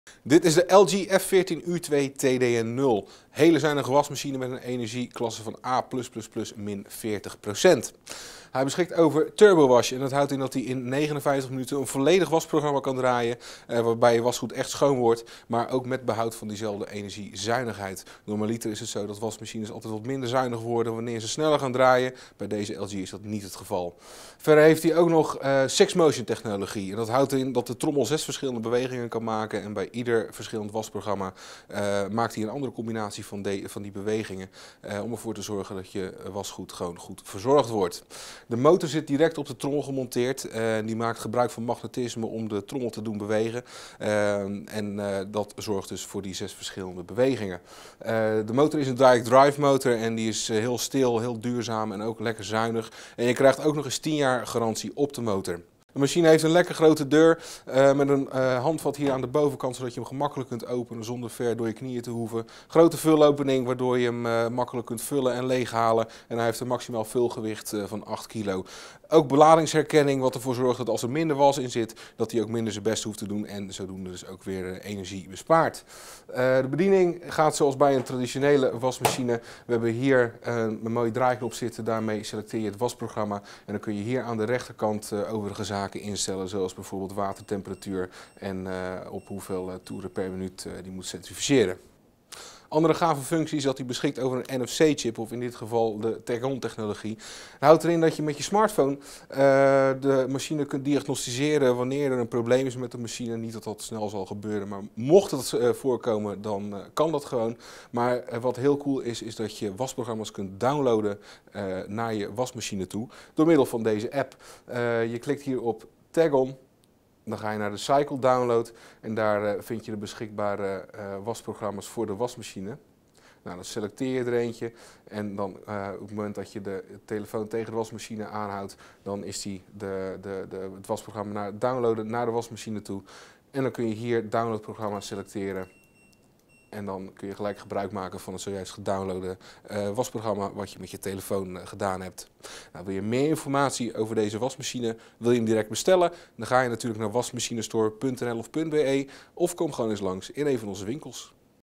LG F14U2TDN0 Wasmachine productvideo (NL/BE)
De LG F14U2TDN0 bestellen? Ga naar

De LG F14U2TDN0 wast je kleding met beleid. Dankzij 6 Motion kan de wasmachine zes verschillende wasbewegingen maken. Afhankelijk van je wasgoed stemt de wasmachine de beweging af. Zo wordt dons of wol heen en weer geschommeld, terwijl katoen krachtiger wordt aangepakt. Bedienen van de LG F14U2TDN0 gaat eenvoudig via het touchscreen. En door simpelweg gebruik te maken van je telefoon is het mogelijk nieuwe wasprogramma's te downloaden. Zo installeer je eenvoudig speciale programma's voor babykleding of wol.